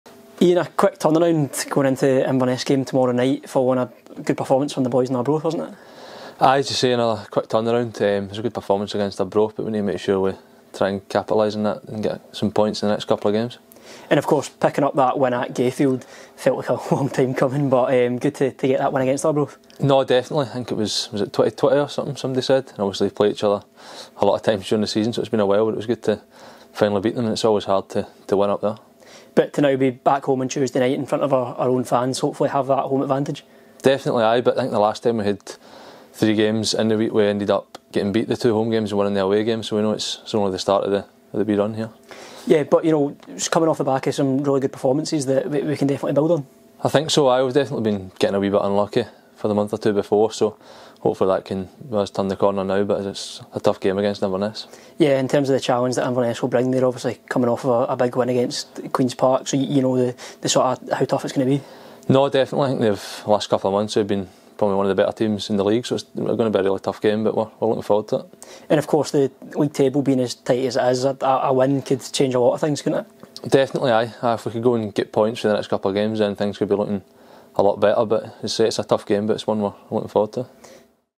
Greenock Morton | Iain Wilson | Inverness C.T. Preview
Hear from Iain Wilson as he gives his thoughts ahead of tomorrow night's match against Inverness C.T.